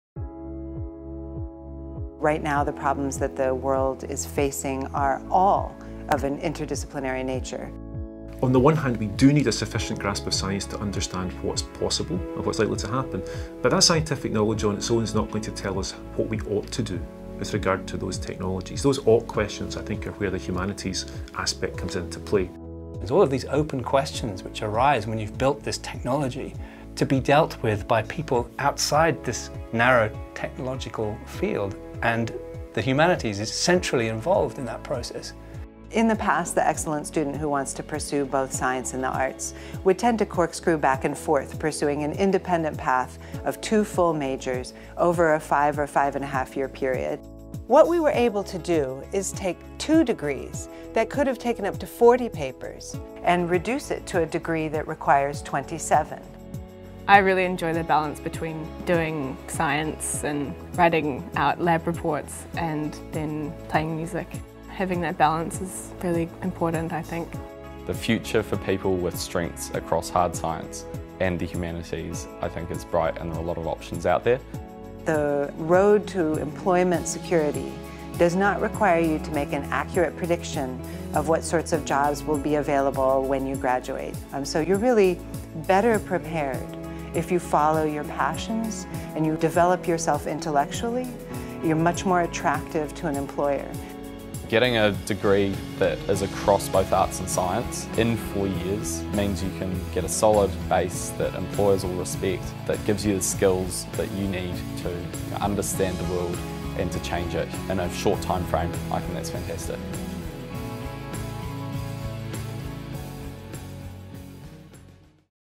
Bachelor of Arts and Science (BASc) degree
Many secondary students are good at both Arts and Science subjects, and feel they have to make a choice between the two when they go to university. No longer! – Otago now offers a combined Bachelor of Arts and Science (BASc) degree.